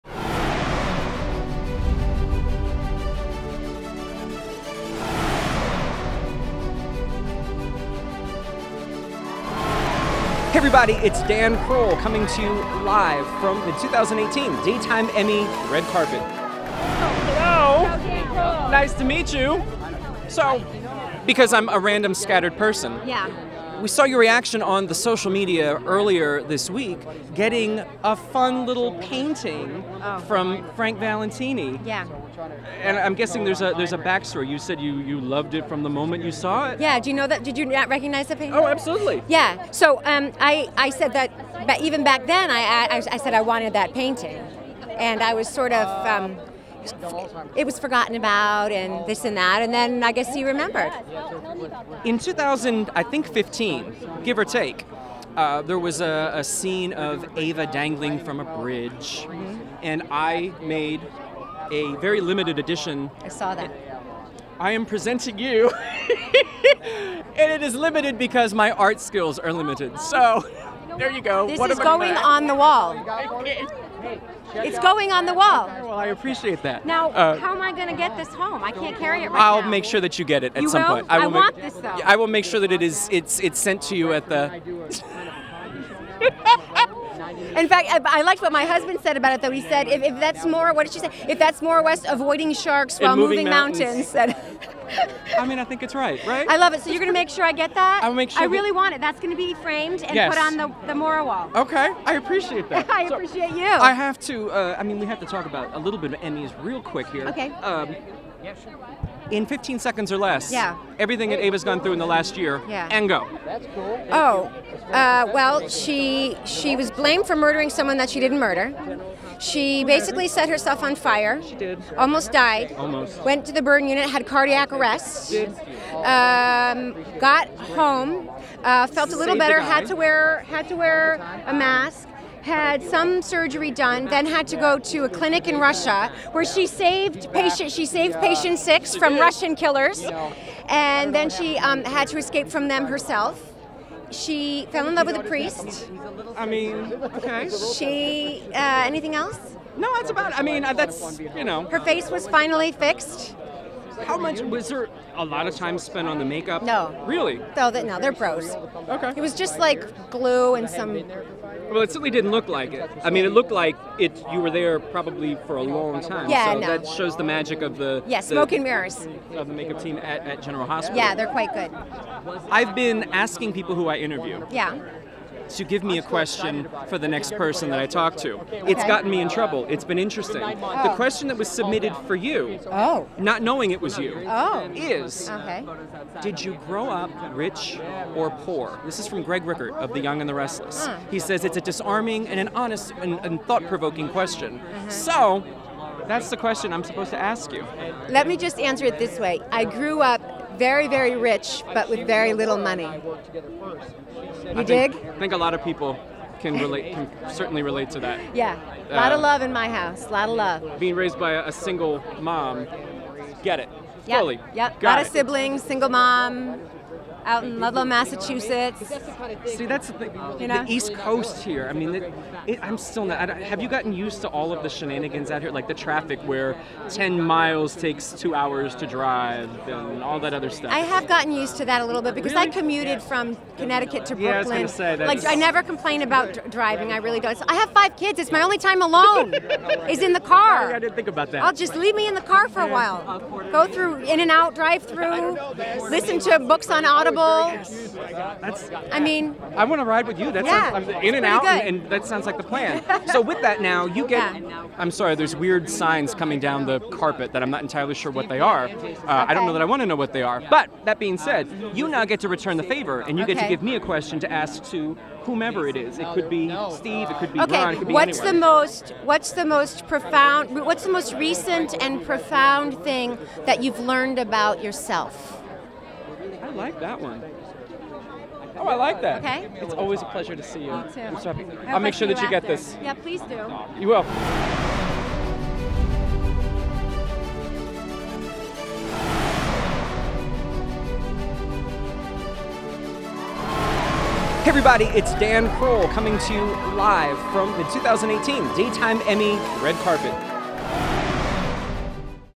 Daytime Emmys 2018: General Hospital's Maura West
Soap Central's Red Carpet coverage of the Daytime Emmys was hosted by Dan J Kroll.

The 45th Annual Daytime Emmys were held Sunday, April 29, 2018, at the Pasadena Civic Auditorium in Pasadena, California.